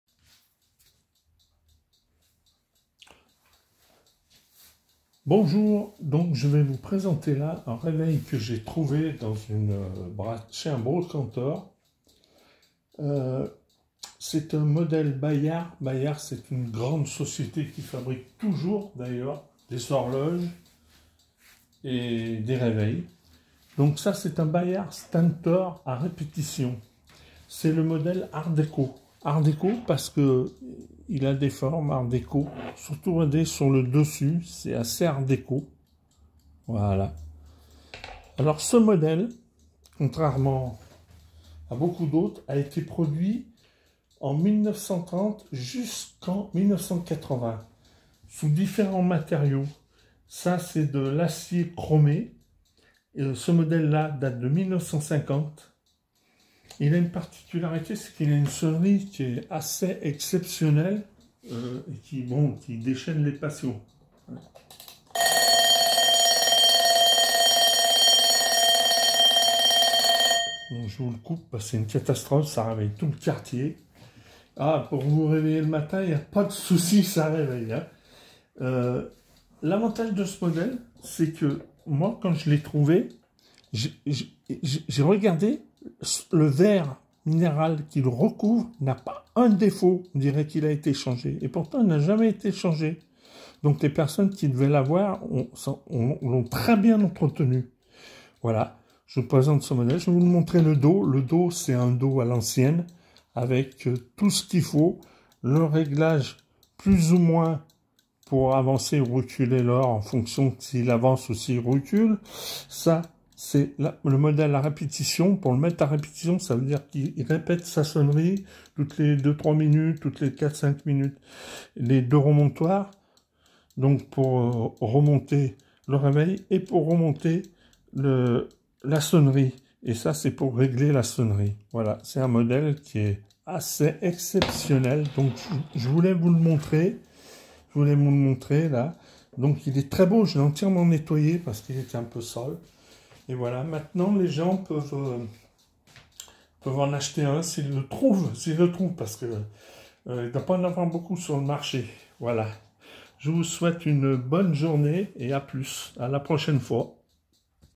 reveil bayard 1950
réveil bayard stentor à répétition de 1950 style art déco.